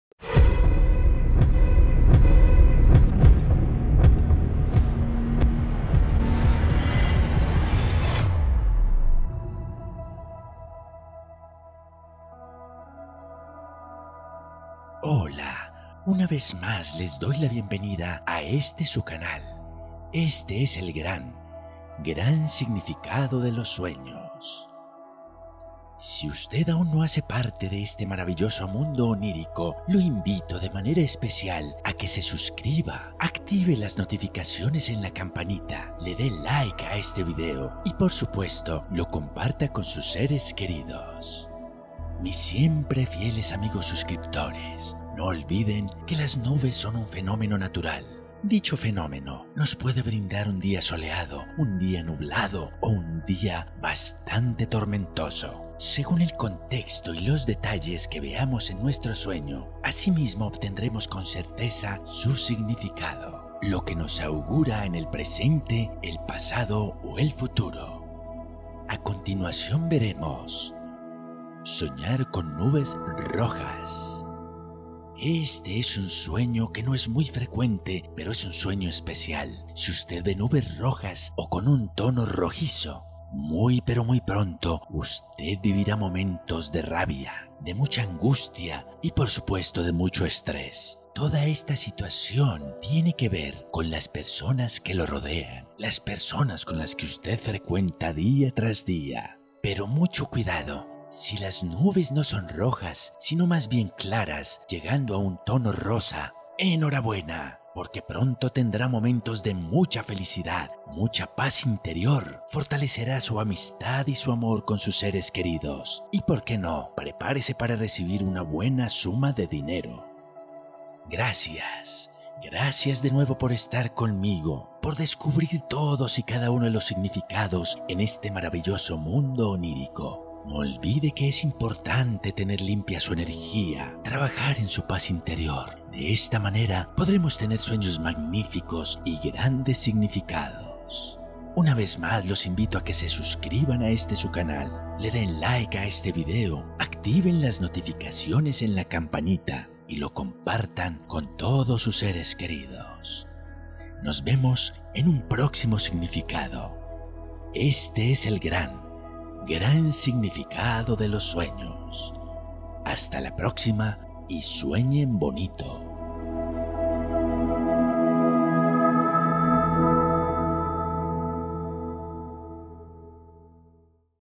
¿QUÉ SIGNIFICA SOÑAR CON NUBES ROJAS? | Significado de los Sueños - SOÑAR CON NUBES, Nubes Rojas ☁️
SOÑAR CON NUBES es un sueño recurrente, presagia acontecimientos que llevamos en nuestro ser. Según los detalles de nuestros sueño puede presagiar el comienzo de una nueva vida.

Veamos a continuación el significado de:
SOÑAR CON NUBES ROJAS

Si en su sueño ve nubes rojas en el cielo significa que...

-PREMONICIONES
-SIGNIFICADO